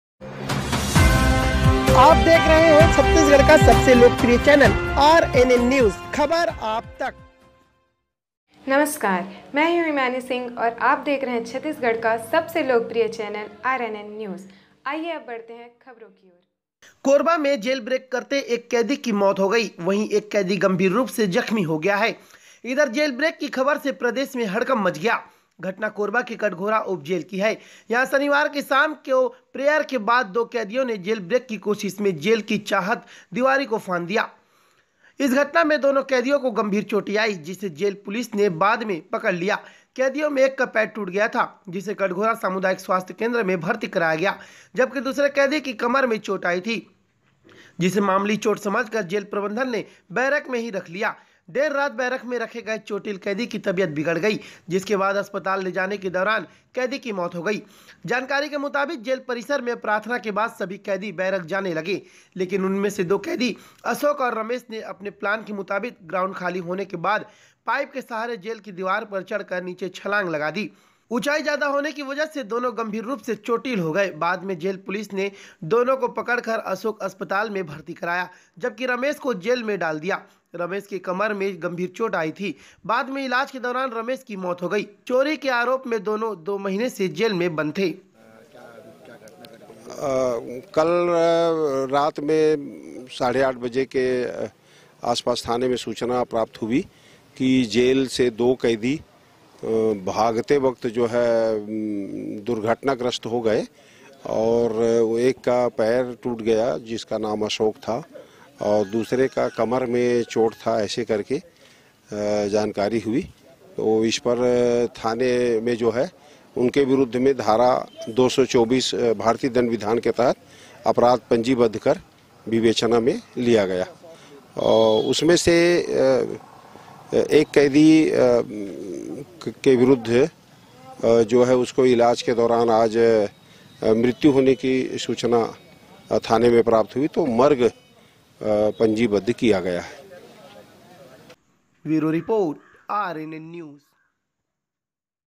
कोरबा/जेल ब्रेक करते एक कैदी की हुई मौत, एक गंभीर रूप से घायल.
Our

The Media News Office,
Near SBI Bank Ompur Rajgamar Colony,
Pin No-495683, District-KORBA(C.G.)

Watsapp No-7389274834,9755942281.